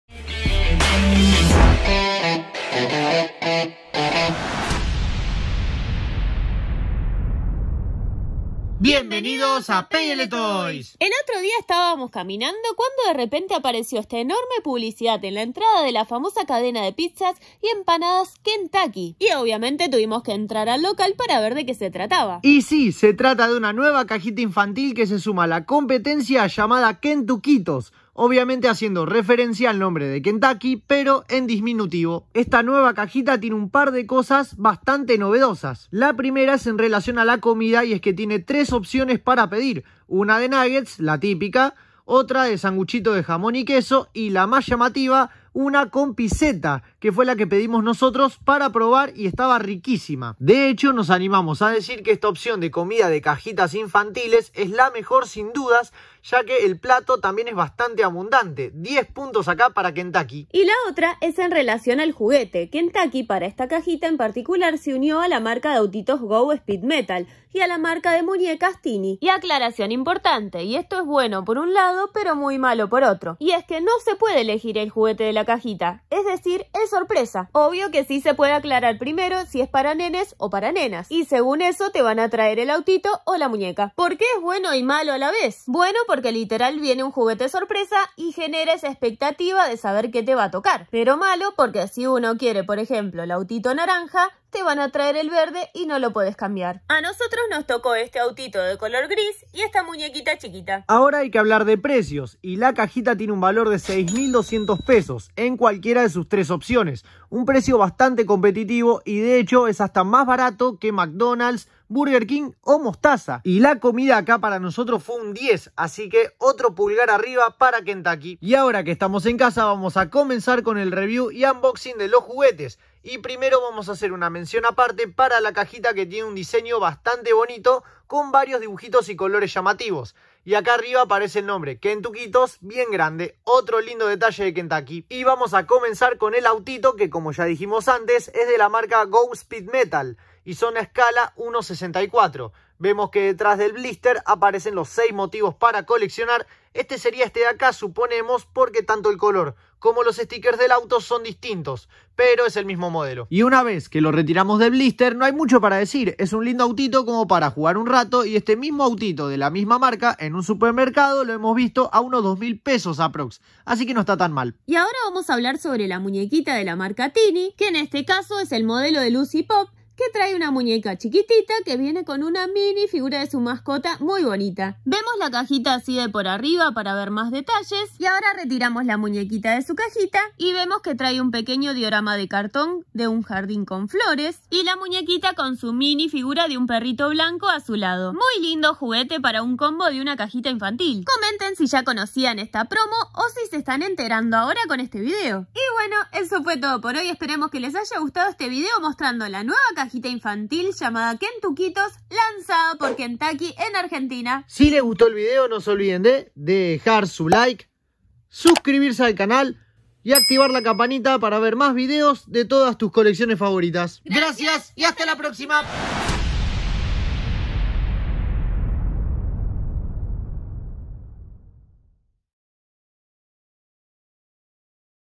¡Kentucky lanzó su Primera Cajita Infantil con JUGUETES!
En julio/agosto la famosa cadena de pizzas y empanadas Kentucky lanzó por primera vez una cajita infantil con juguetes y obviamente tuvimos que ir a ver de qué se trataba.

No se olviden de seguirnos en Instagram para ver más juguetes y novedades